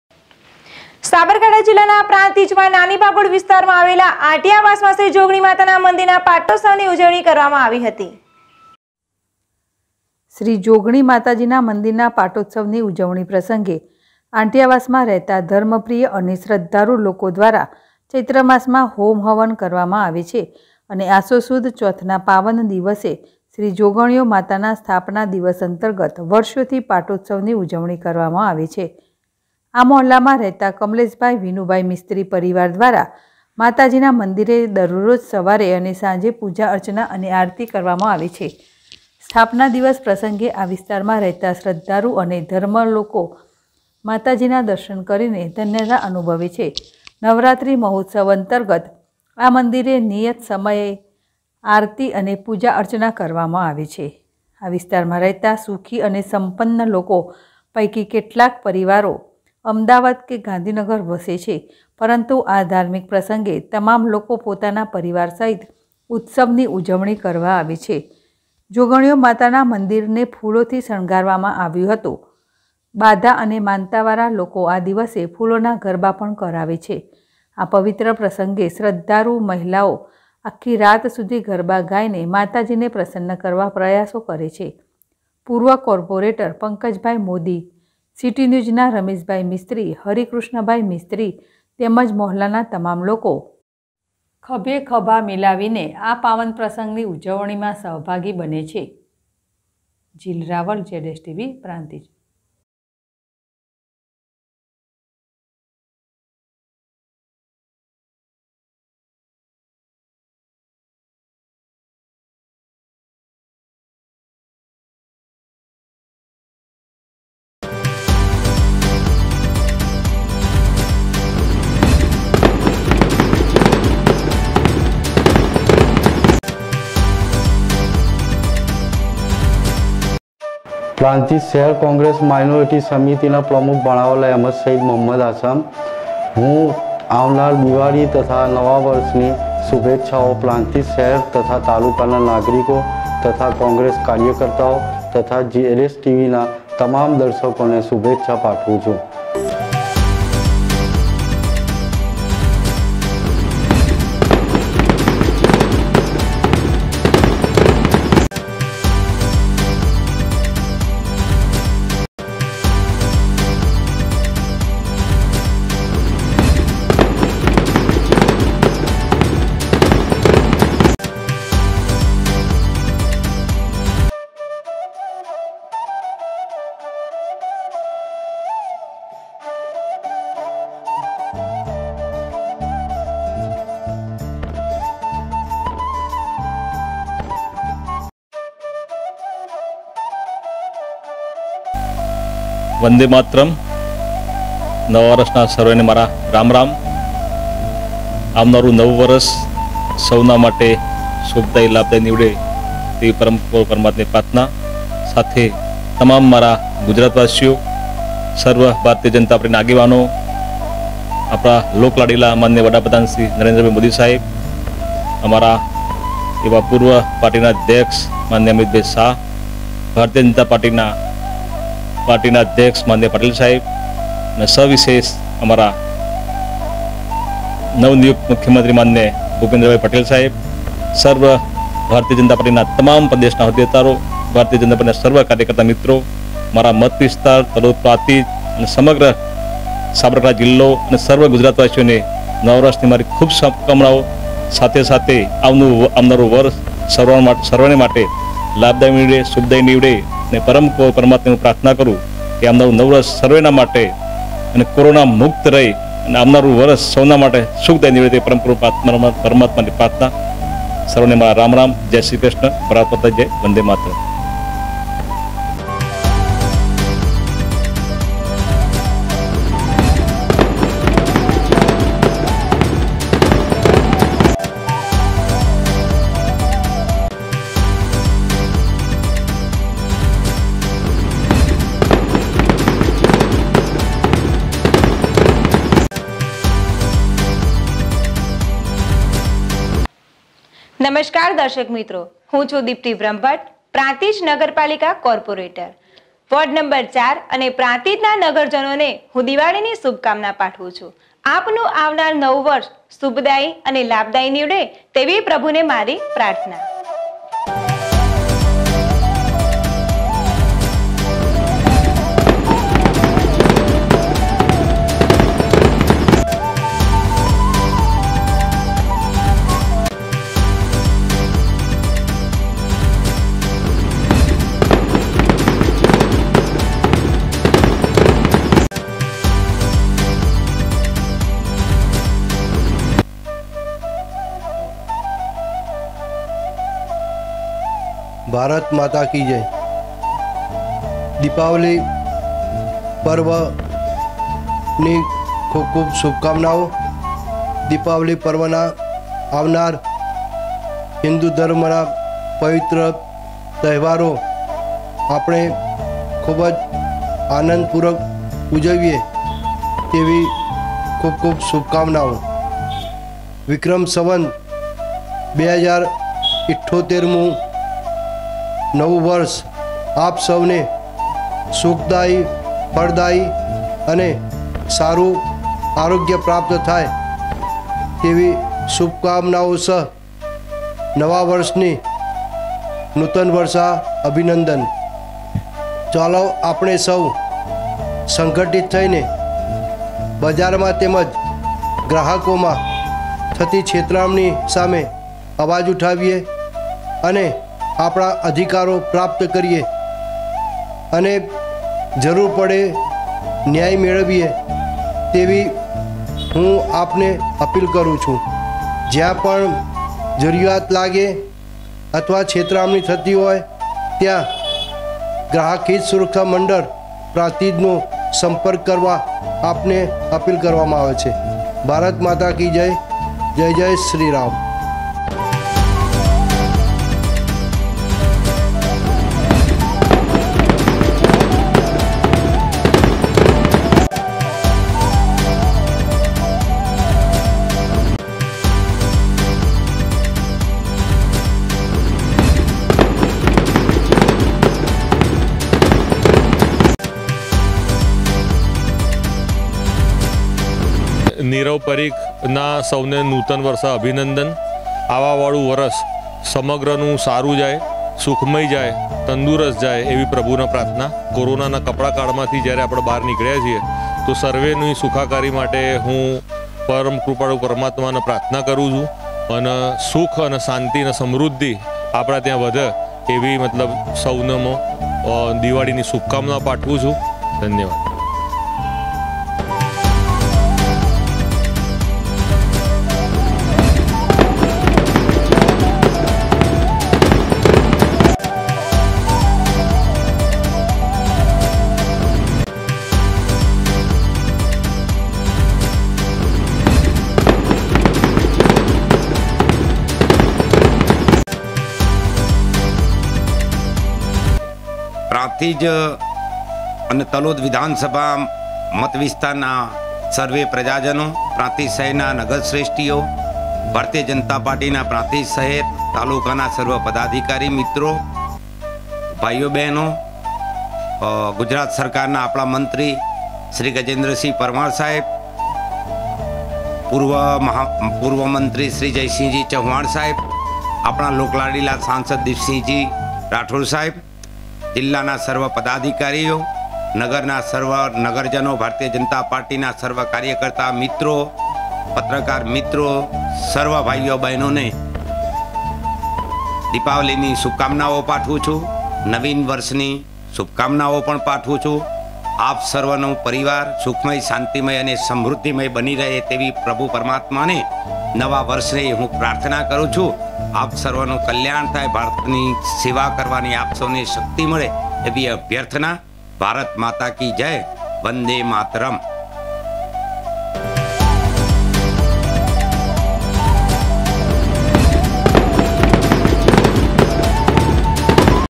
પ્રાંતિજના આંટીયાવાસ માં શ્રી જોગણી માતાજીના પાટોત્સવ ઉજવણી કરવામાં આવી
પ્રાંતિજના આંટીયાવાસ માં શ્રી જોગણી
 માતાજીના પાટોત્સવ ઉજવણી કરવામાં આવી//ZSTVNEWS//